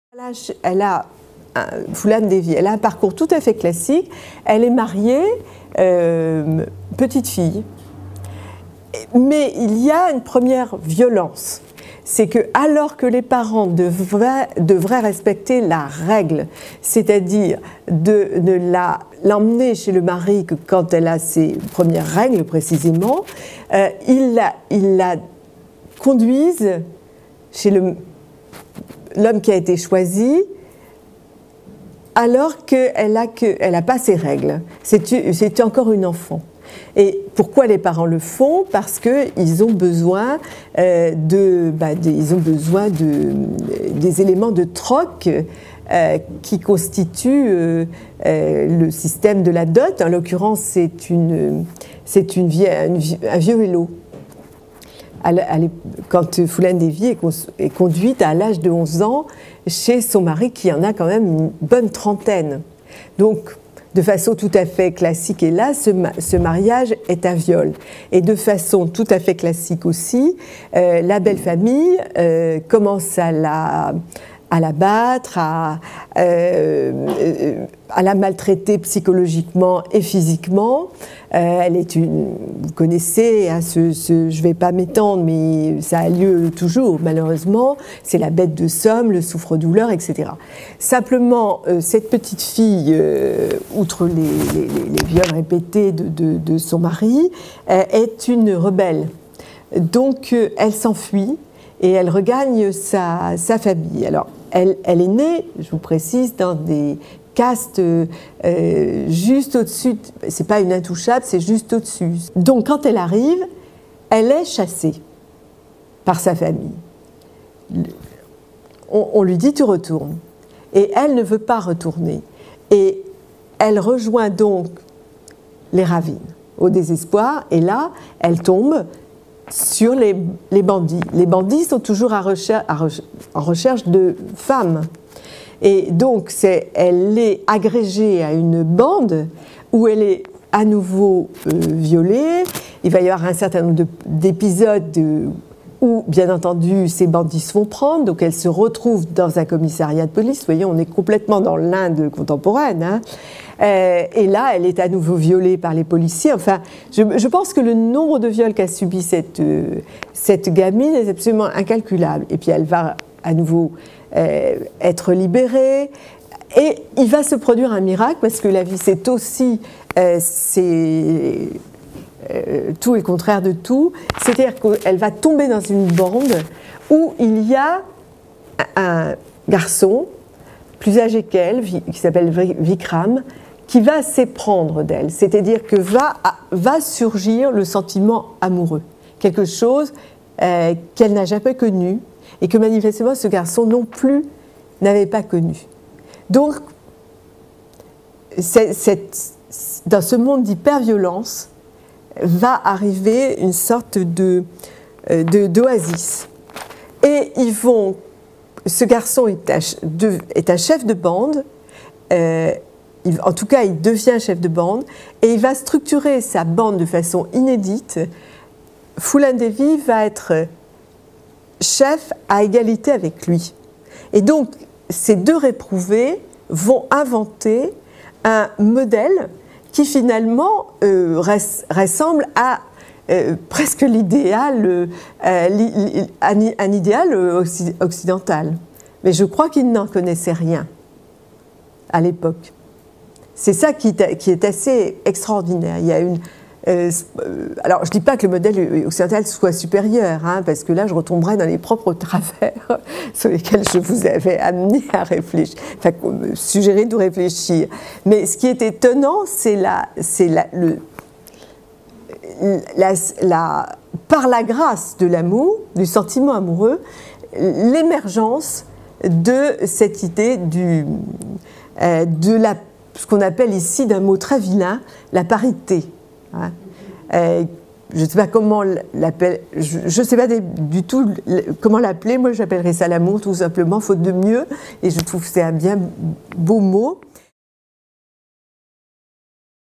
Phoolan Devi et Vikram de l'amour à la parité
d'un exposé intitulé: Figures de femmes indiennes, par: Irène Frain